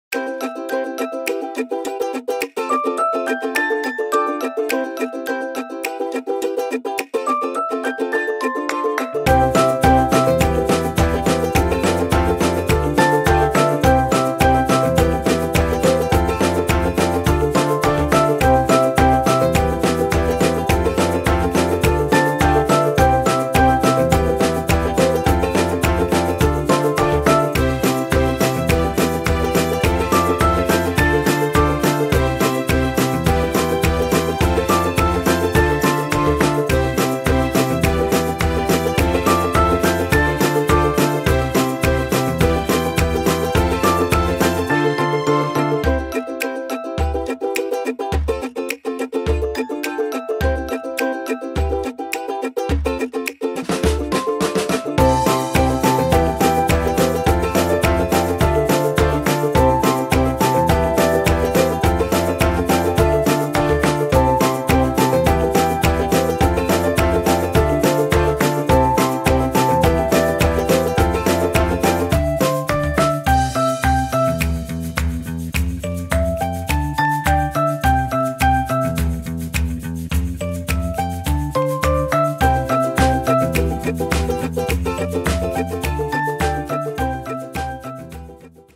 Behind the Scenes with Nelydia Senrose
Catch a glimpse of what went on during the mix and match fashion shoot with Nelydia Senrose.  Plenty of preparation, organisation, sweat and make-up going on.  It’s not easy when you prepare a shoot outdoors. Happy watching girl, and you too, boys!